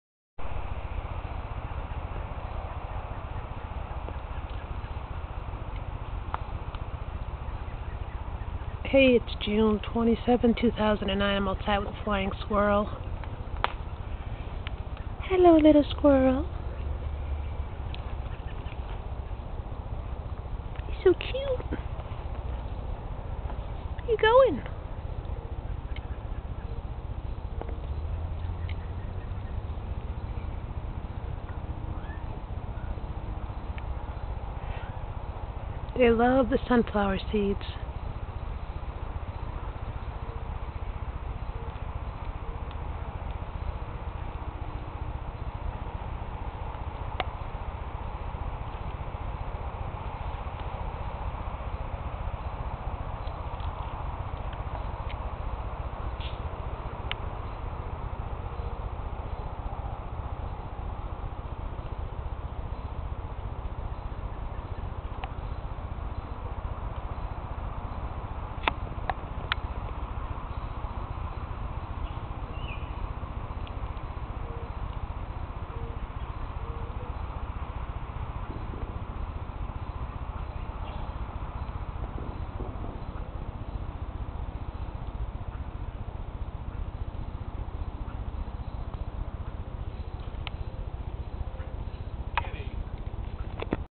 Northern Flying Squirrel in my backyard ~ Bellingham Massachusetts
I enjoy the Flying Squirrels a lot. This is the first time I got a video of one,, they are pretty fast.